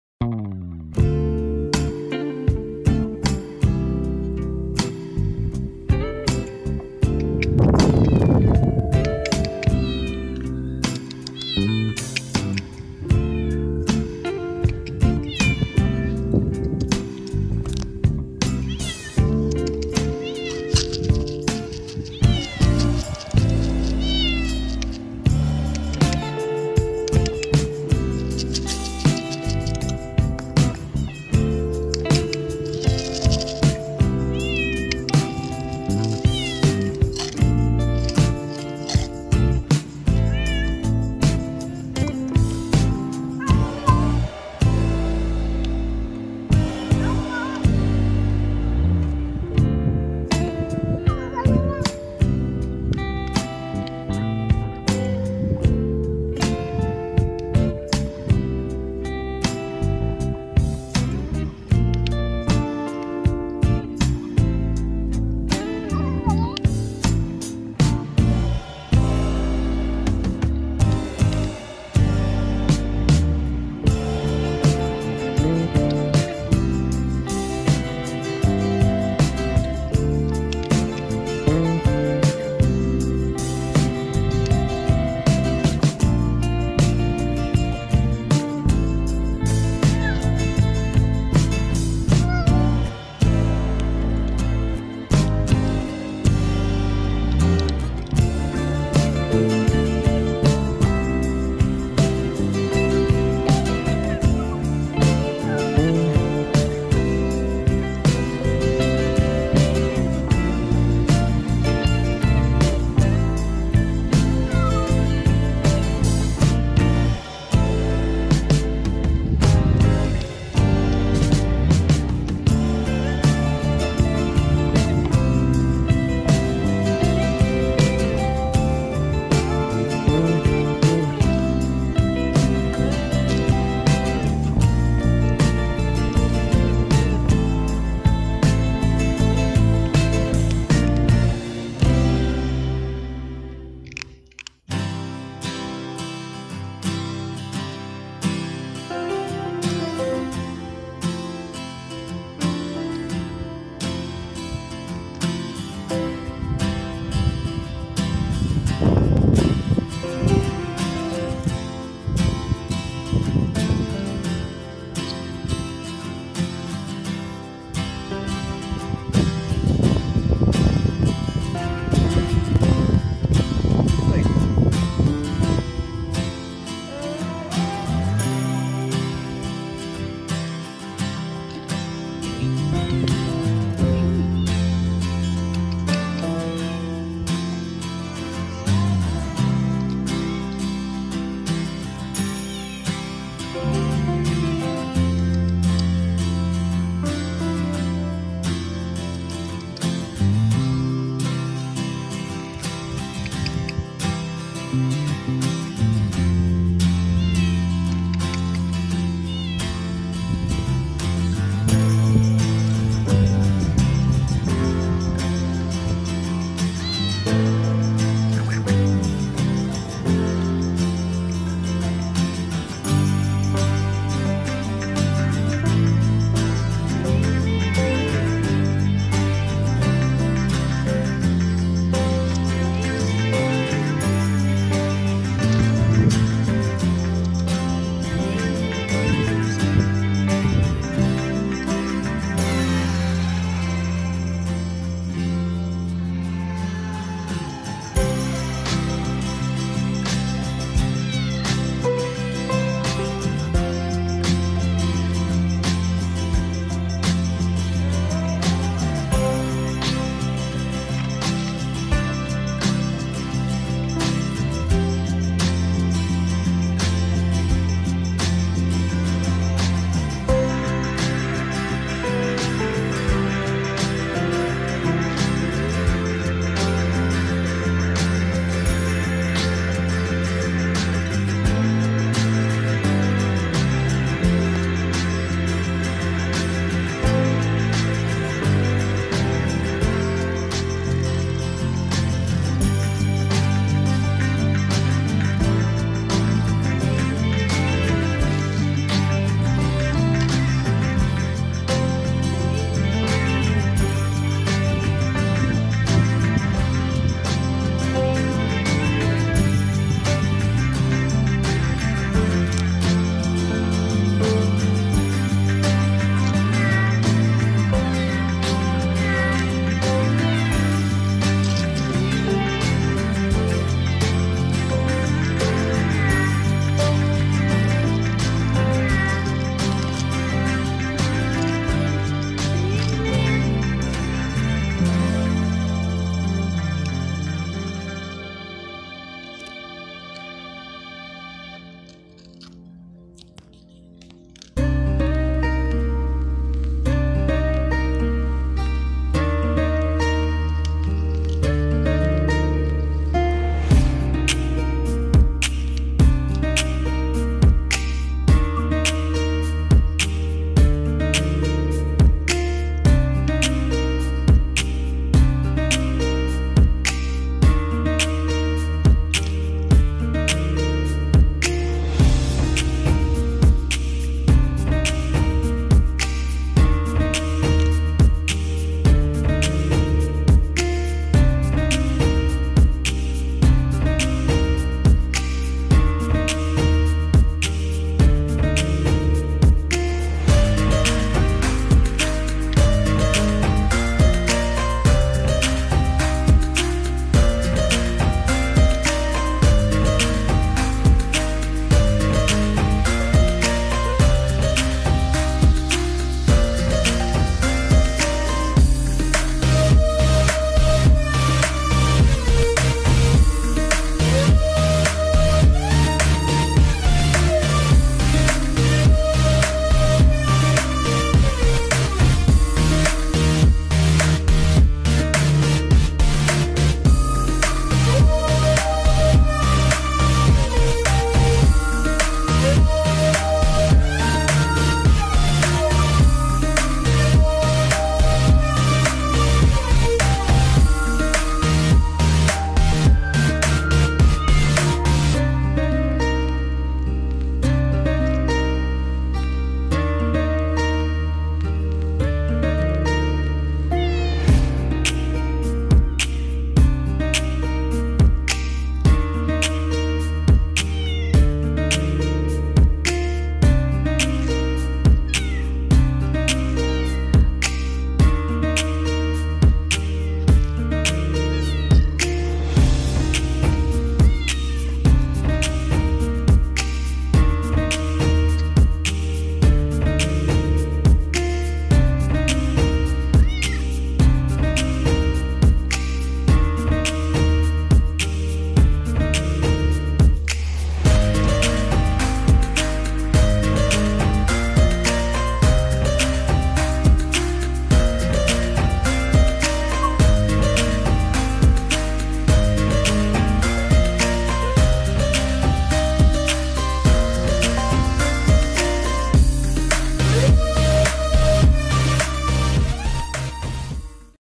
Amazing Cat Meowing 120% so Strong so Loudly
Amazing Cat Meowing 120% so Strong so Loudly. Today I always went to find that kitten 🐈, now almost adult, a male who seems to be in heat but in reality his meow is referred to his abandonment. In fact, his master probably abandoned him in a garden where he therefore lives alone and homeless. Feral street abandoned and alone cat meowing for food and pets every day. Even today I brought treats 🍲 and gave him many caresses that he loves. God bless you and see you tomorrow. Enjoy with Funny Kittens and Cats Videos channel 😻.